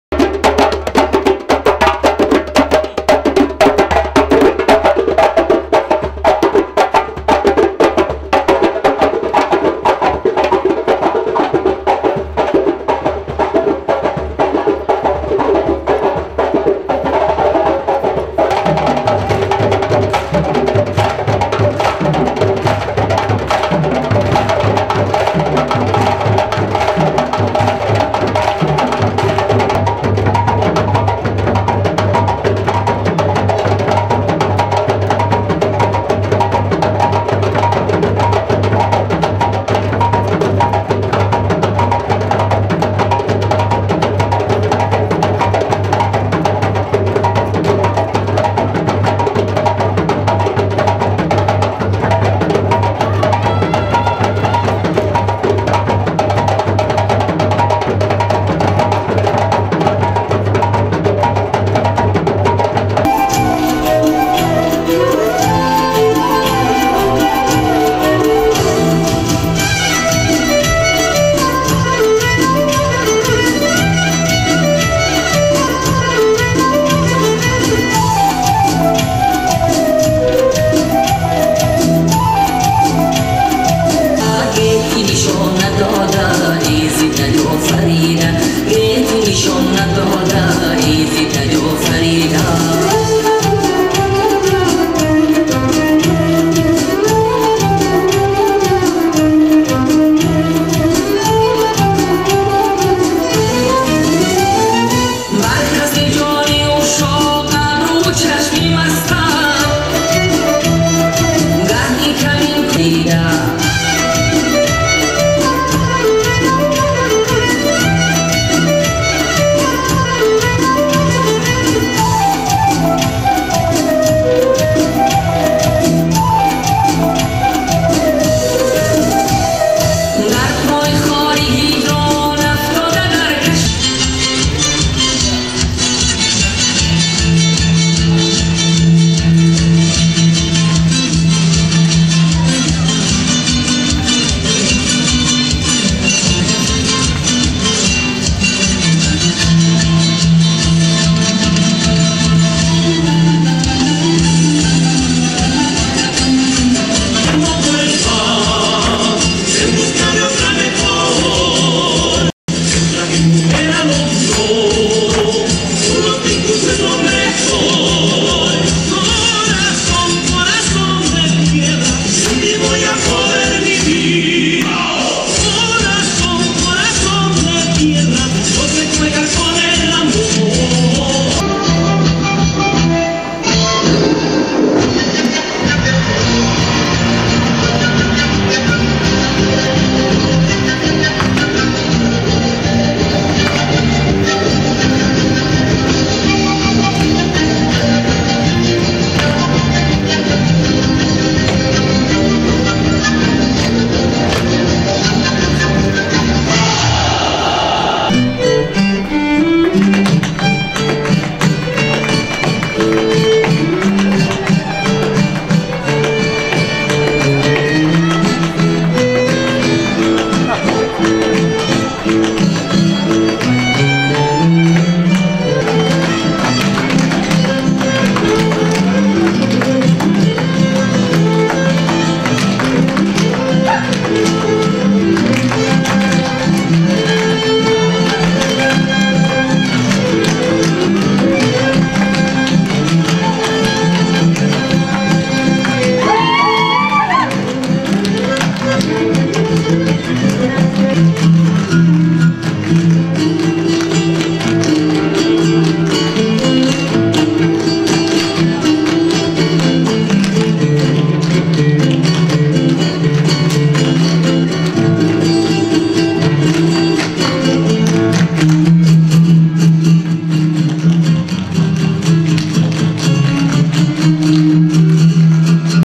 Celebration of Dance
The NCIV 50th Anniversary Gala featured "A Celebration of Dance," with performances by four world dance companies.  Gala participants were treated to  performances by Kankouran West African Dance Company, Silk Road Dance Company, Alma Boliviana, and Footworks Percussive Dance Ensemble.